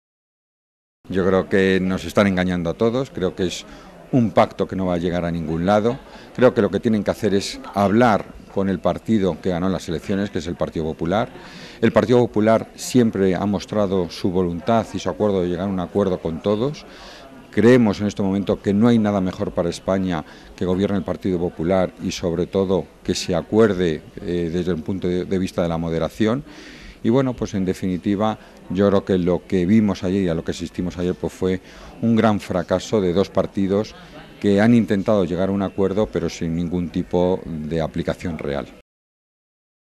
“PSOE y Ciudadanos nos están engañando”
El consejero de Servicios Sociales de la Comunidad de Madrid se ha mostrado así de contundente respecto al acuerdo entre ambos partidos